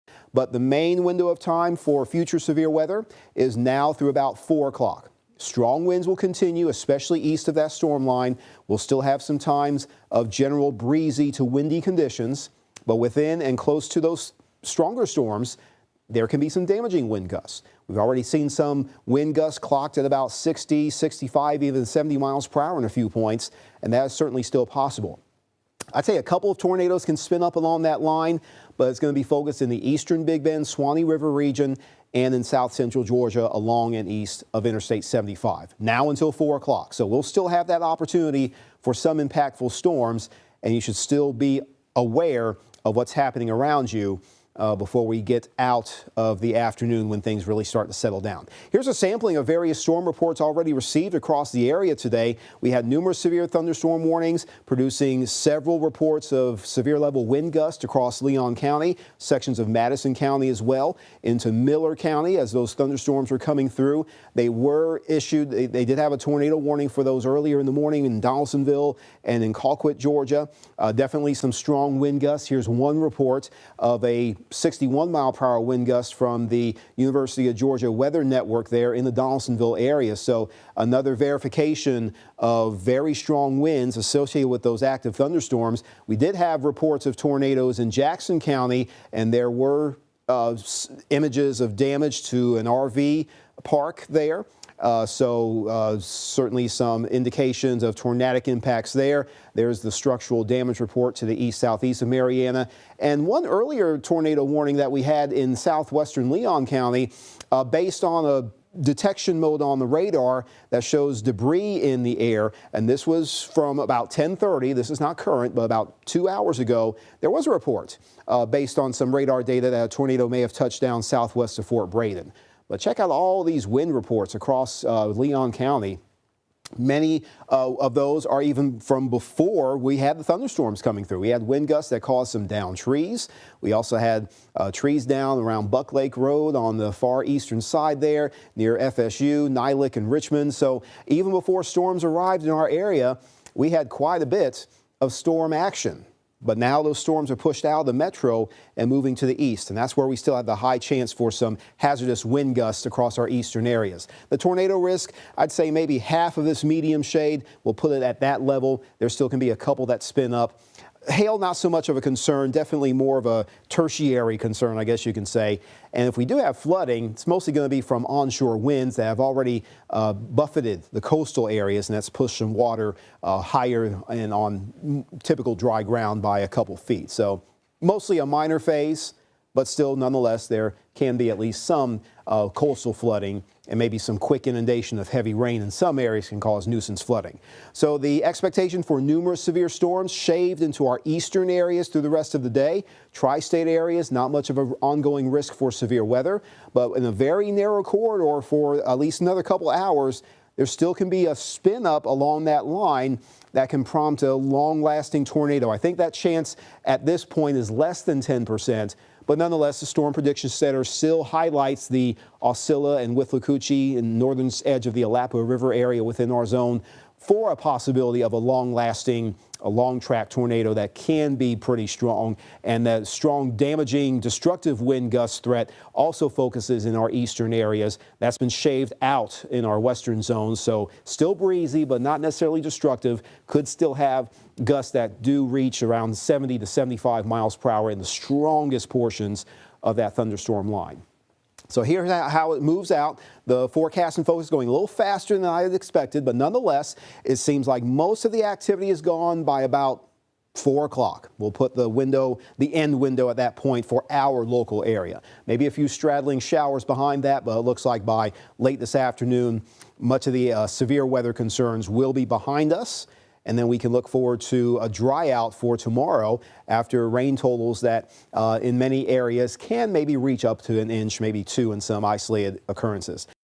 Severe weather hits Big Bend and South Georgia: Tuesday afternoon update
Severe weather in the form of damaging winds and intense thunderstorms hit Big Bend and South Georgia on Tuesday morning.